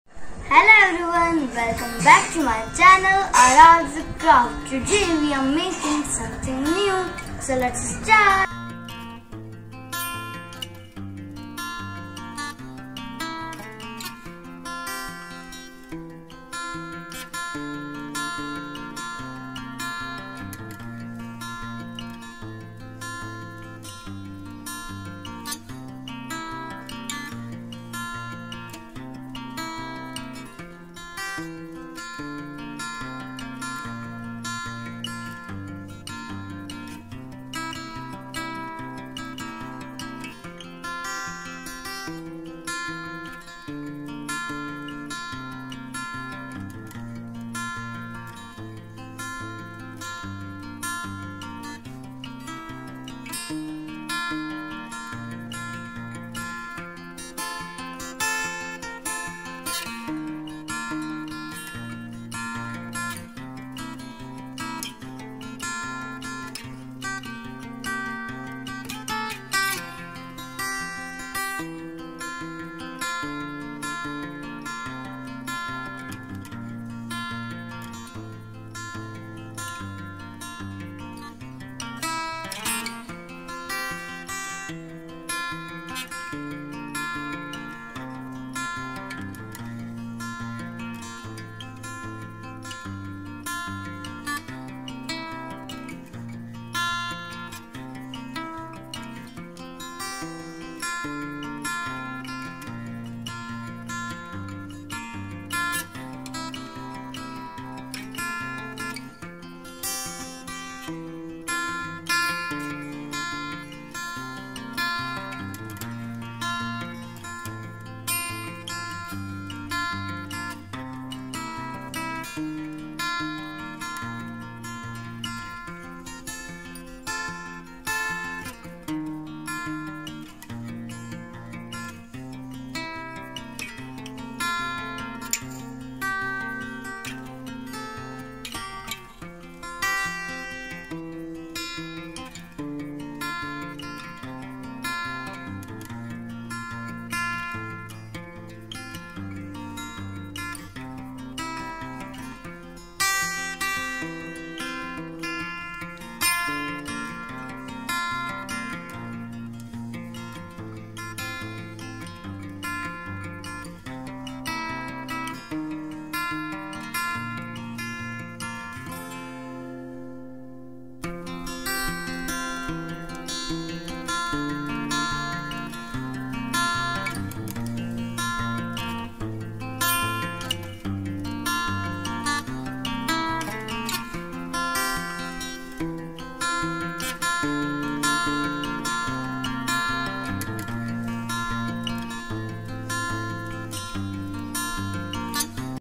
How to make Realistic ,Easy paper rose |Paper Flower DIY||Aaradhy's Craft
Beautiful 3D Paper Rose. How to make easy paper rose.
Easy DIY
Easy Room Decor

Supplies

1. Coloring Paper/Craft Paper
2. Glue
3. Scissor etc.